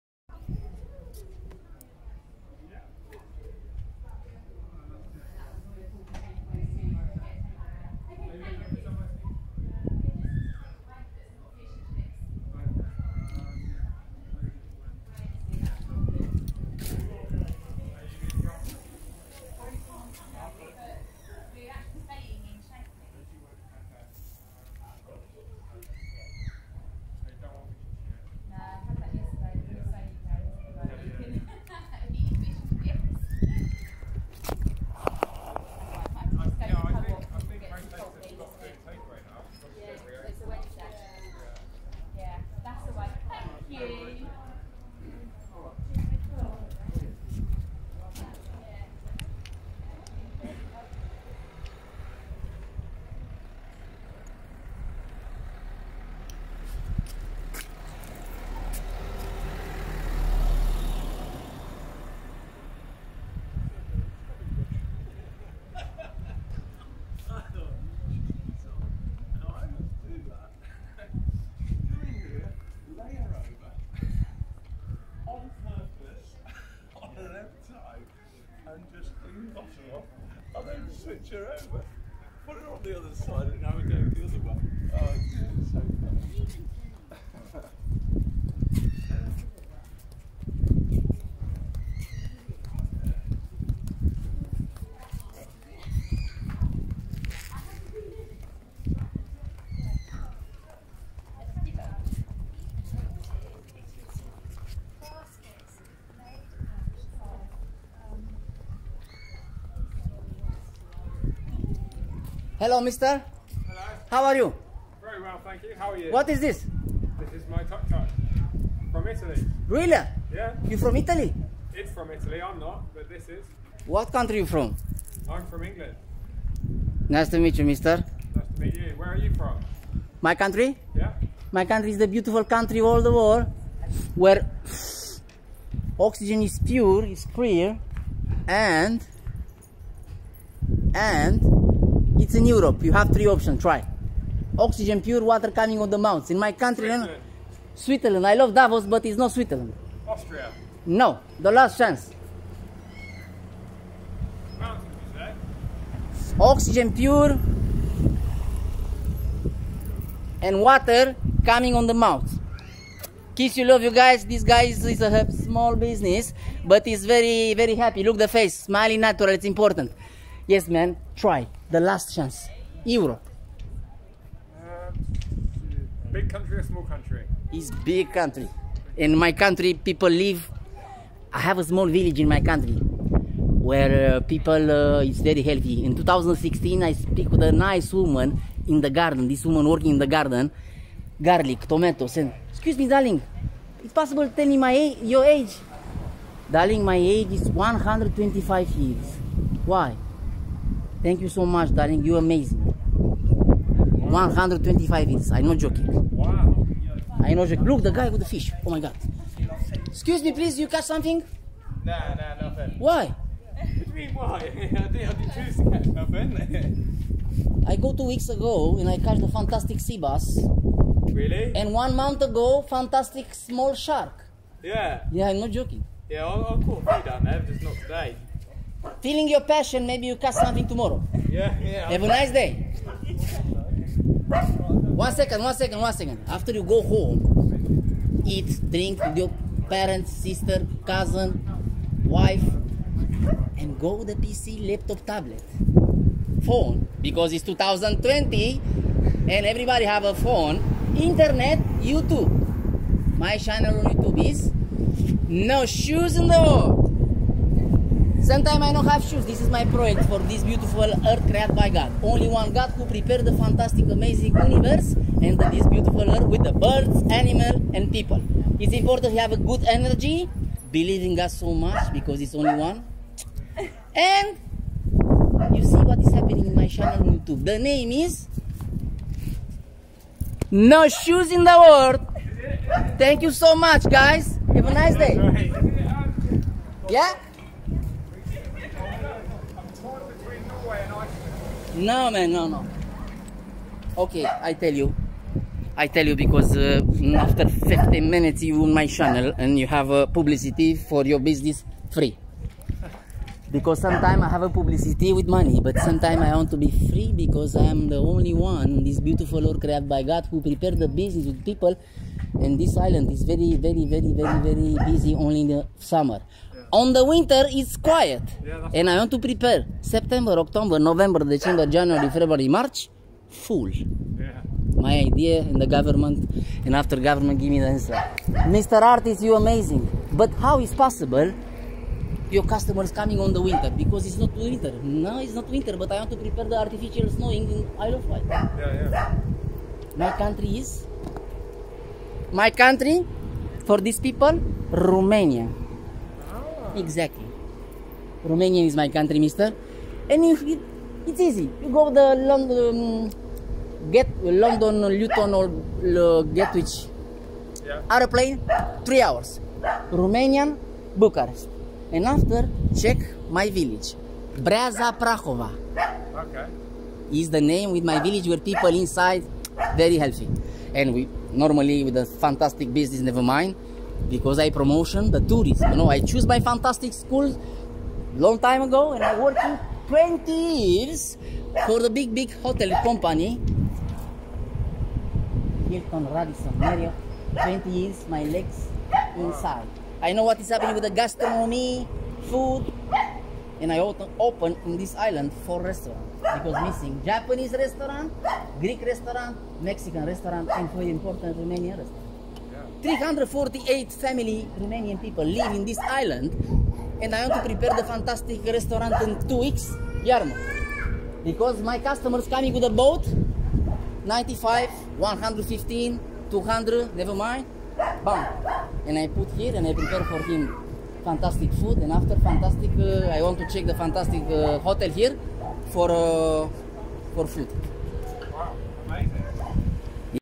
Fantastic Place Isle Of Wight /Yarmouth /UK
Good Service, Good Food and Nice Wine in,, The George Hotel Fantastic Place ⭐⭐⭐ Stars👌🌞😍🇬🇧🇧🇪👆🙏🕗🌎💙🤗❤️